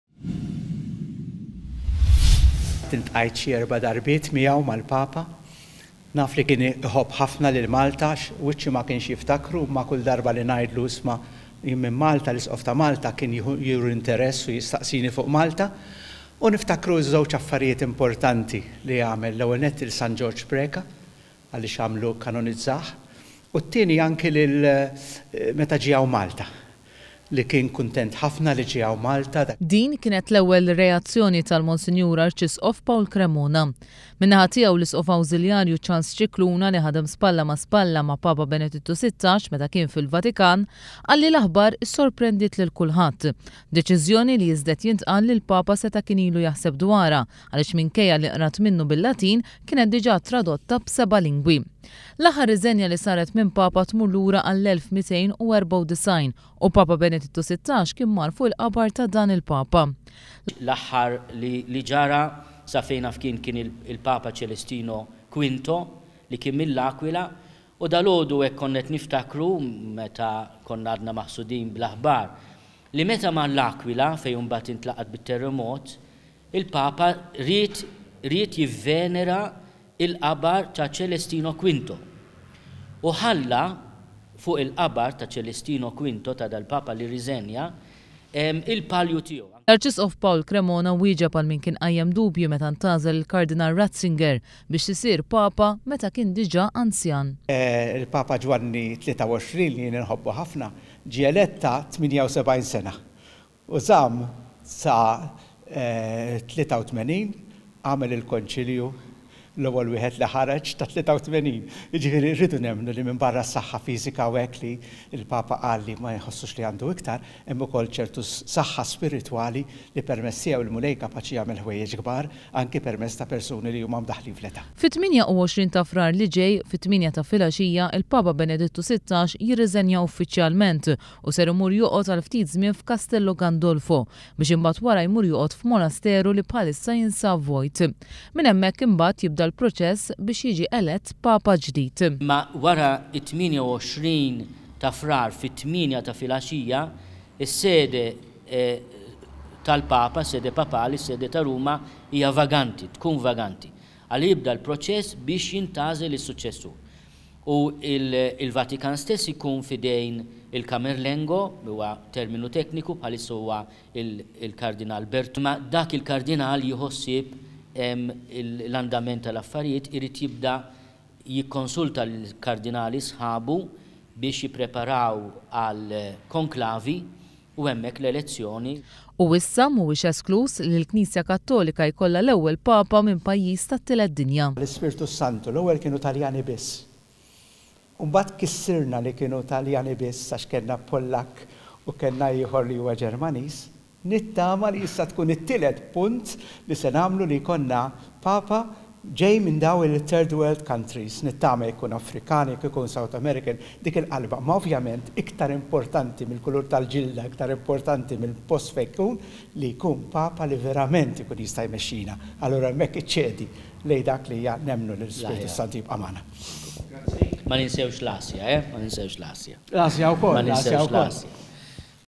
Il-Papa Benedittu Sittax se jibqa mfakkar għall-kanoniżżazjoni ta' San Ġorg Preca
Il-Papa Benedittu Sittax se jibqa mfakkar mal-Maltin u mal-Ghawdxin ghall-kanonizzazjoni ta' San Gorg Preca u ghaz-zjara li ghamel f'pajjizna fl-elfejn u ghaxra.Din kienet l-ewwel reazzjoni tal-Arcisqof ta Malta Pawl Cremona f'konferenza tal-ahbarijiet li fiha id-djocesi ta' Malta kkonfermat l-ahbar li l-Papa irrizenja minhabba ragunijiet ta sahha